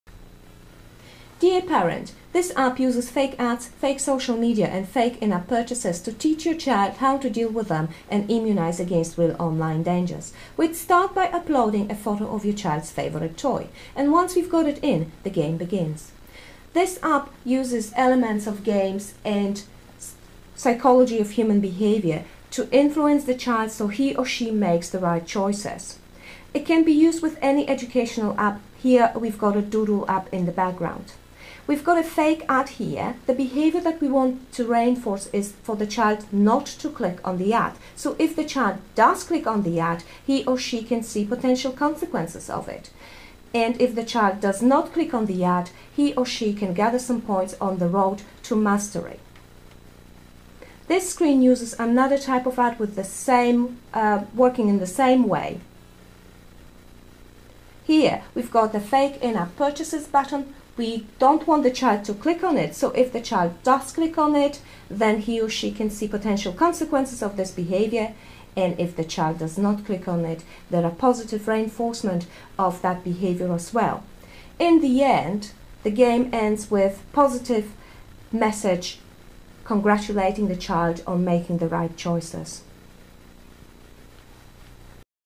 adzjab prototype ver. 1
Have you heard about children spending hundreds of $$$ on in-app purchases? The solution is here!
We teach kids how to avoid in-app purchases and advertising.
This is our prototype.
Our tool can be used with any mobile edu app or game aimed at young children.